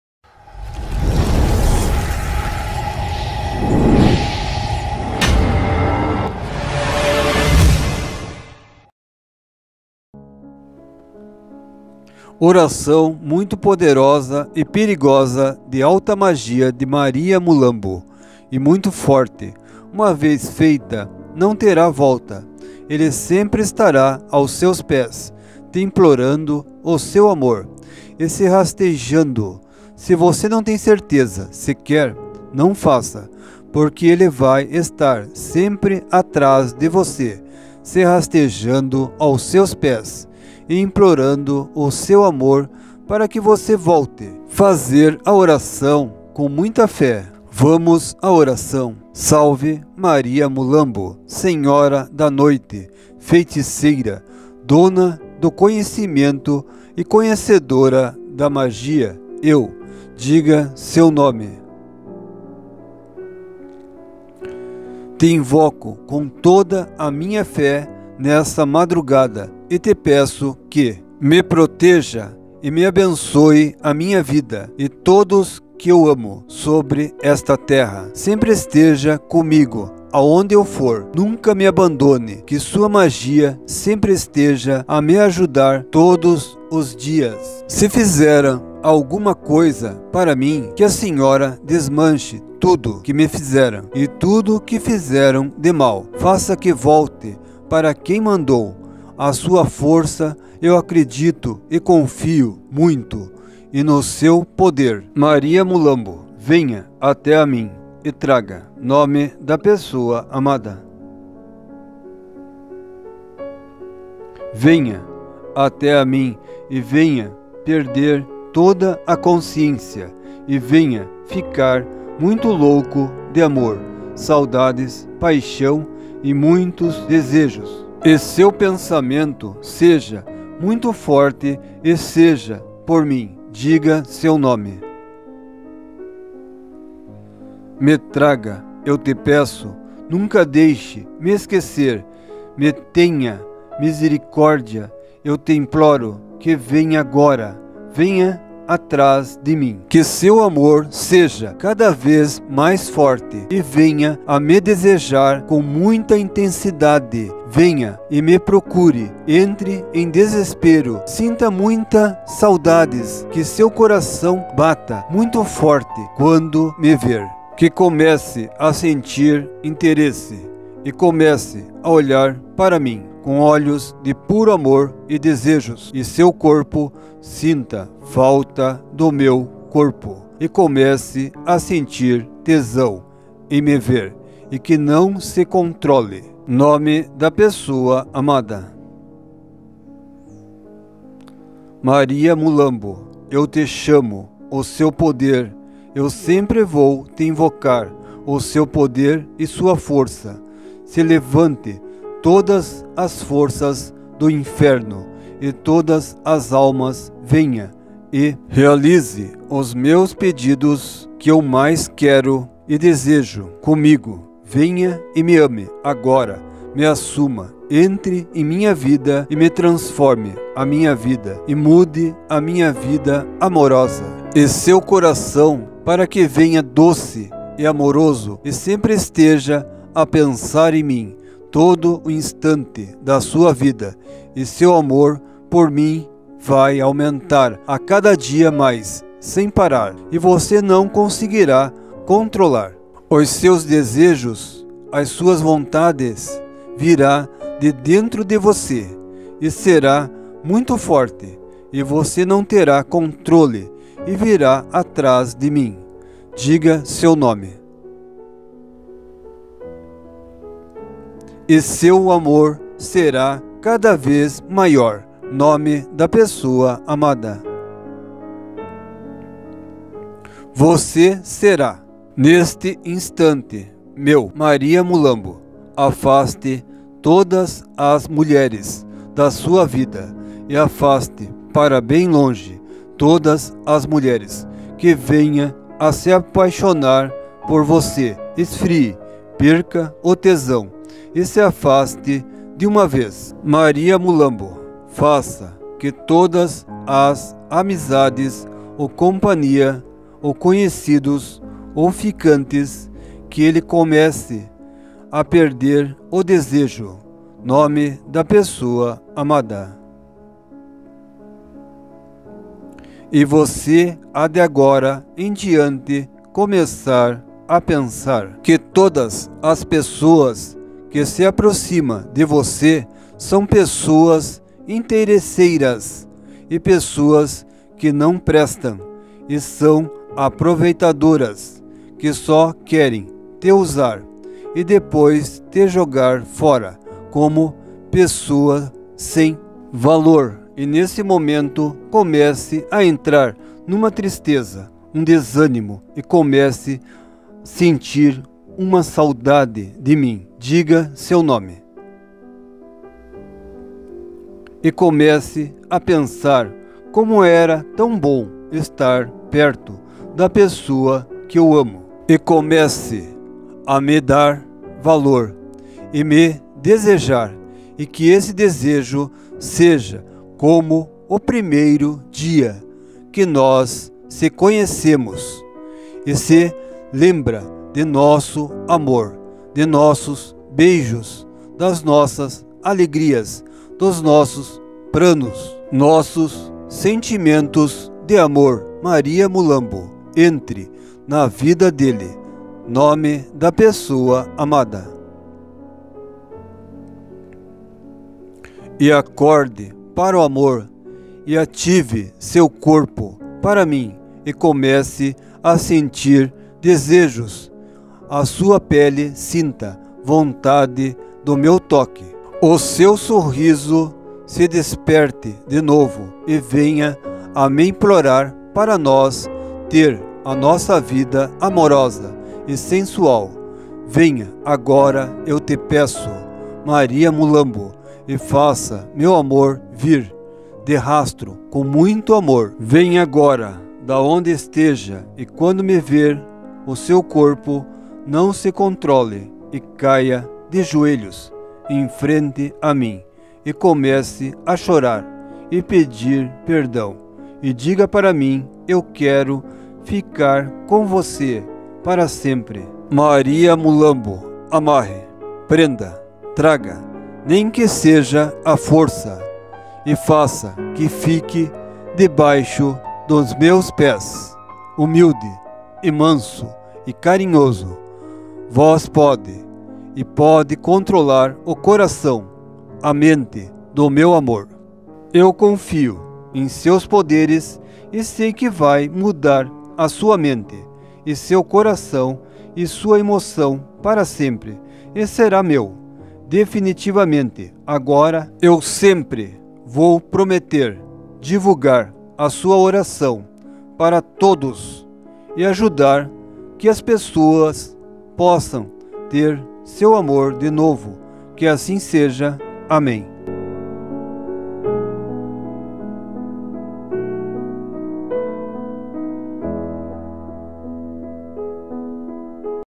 Antiga Oração De Amarração Para Ele Tomar Nojo Dela (Maria Mulambo)
Oração muito poderosa e perigosa de alta magia, Maria Mulambo ,e muito forte, uma vez feita não terá volta ele sempre estará ao seus pés te implorando o seu amor e se rastejando  , se você não tem certeza se quer, não faça , por que ele vai estar sempre atrás de você  se rastejando aos seus pés  e implorando seu amor para que você volte

Fazer oração com muita fé
Vamos a oração